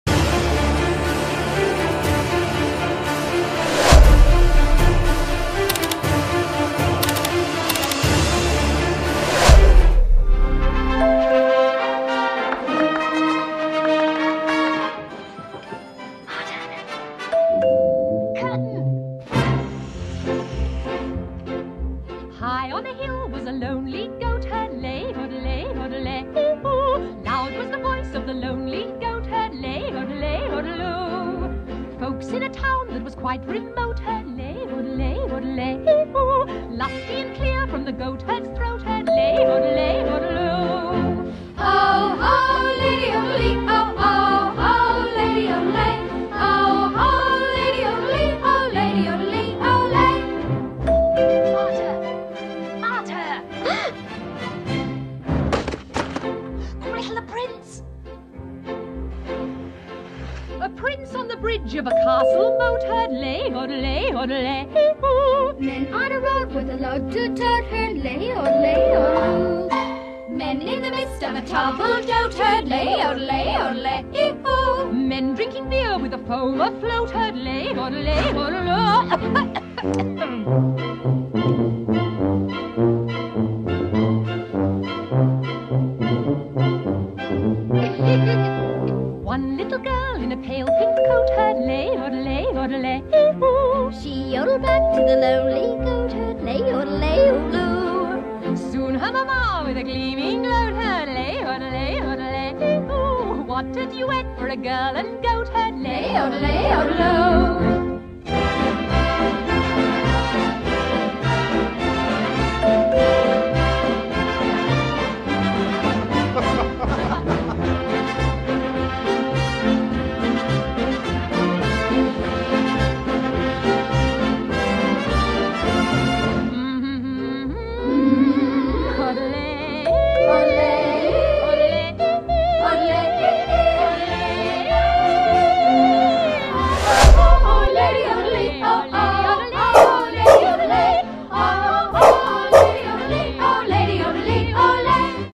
High on Stave Hill looking for my Goat herd
The title will make sense once you watch this drone footage around Stave Hill, South East London on a beautiful sunny afternoon